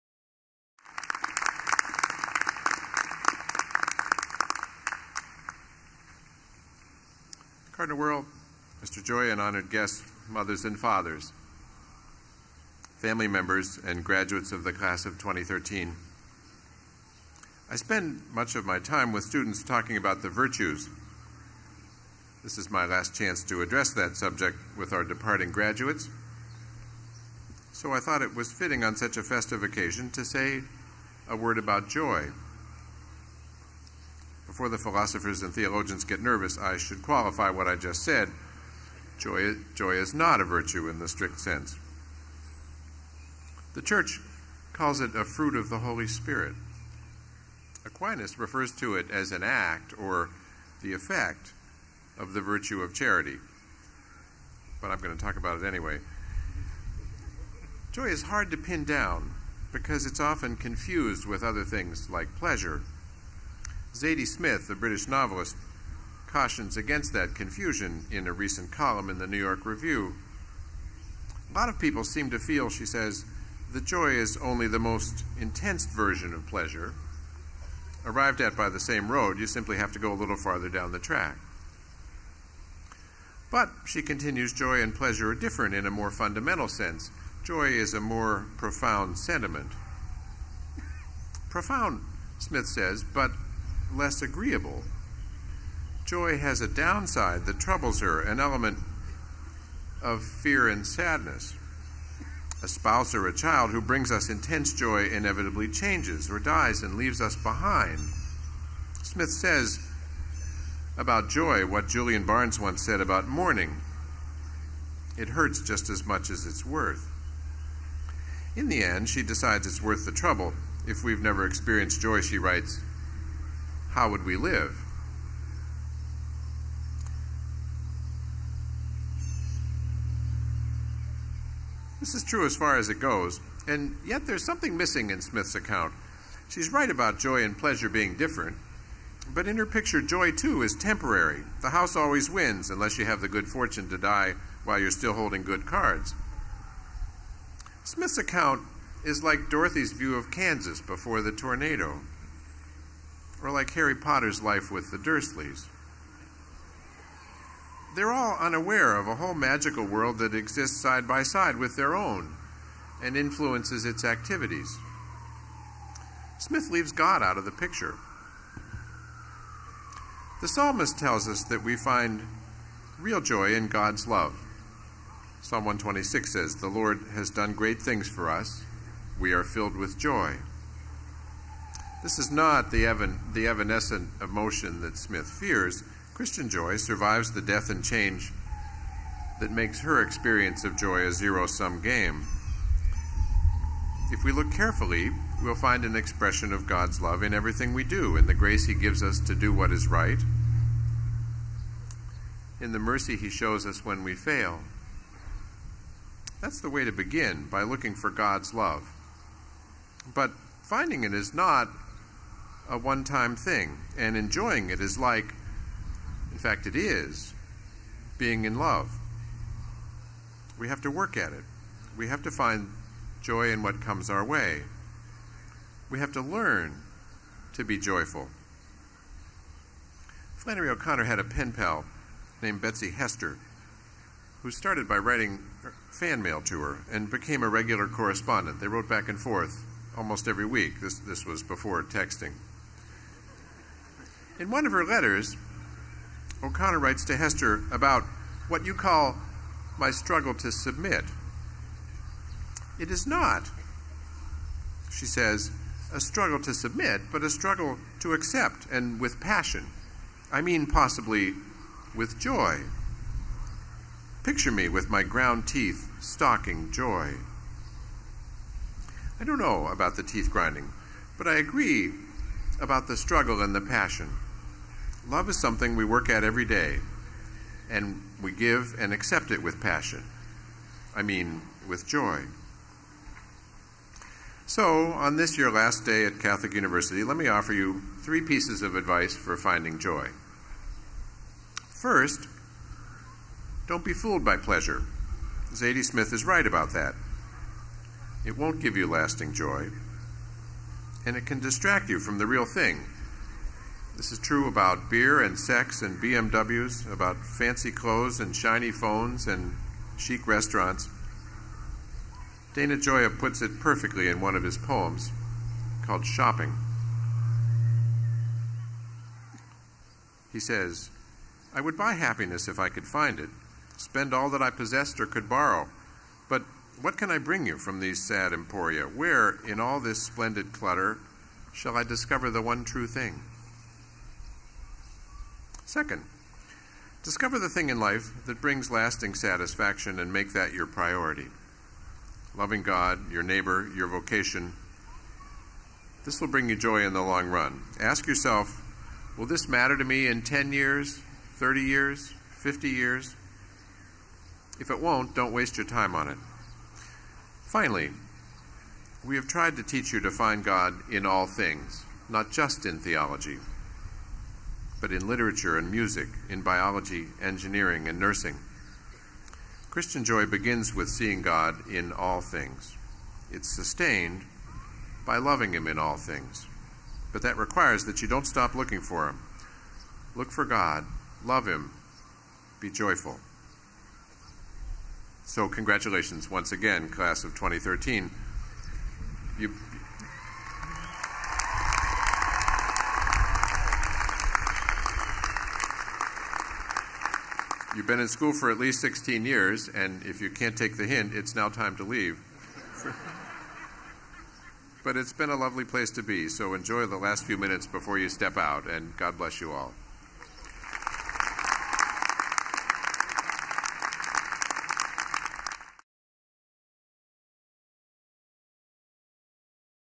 Commencement 2013: University President John Garvey Remarks
President John Garvey addresses the Class of 2013 at the 124th Annual Commencement Ceremony of The Catholic University of America on Saturday, May 18, 2013.